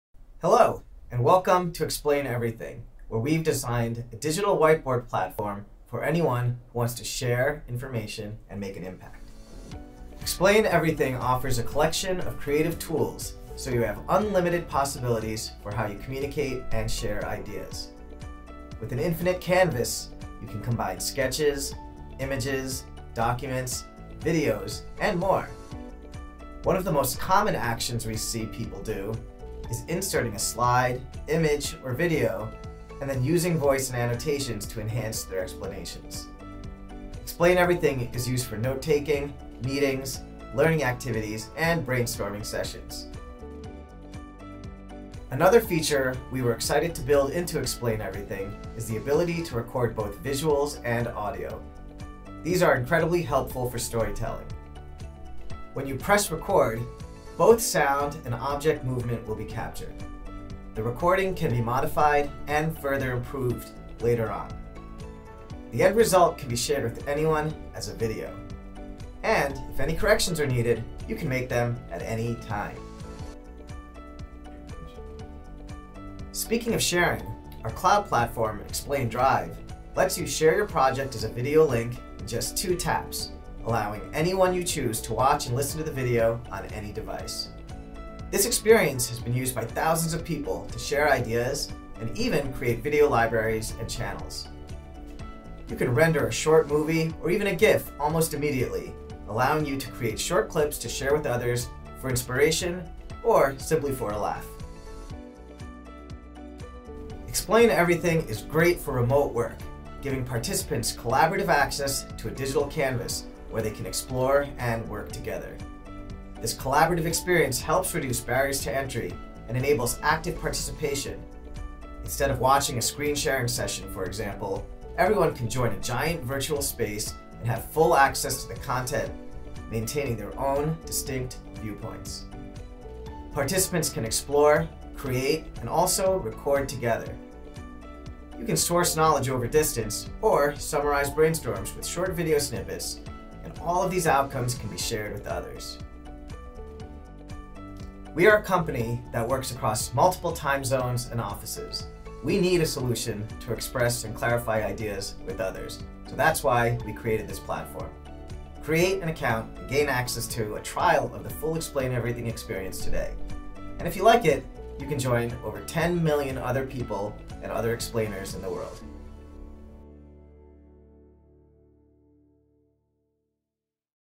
The Tour of Explain Everything Interactive Whiteboard
See how the co-founder of Explain Everything presents four things you can do using our interactive whiteboard in less than 3 minutes.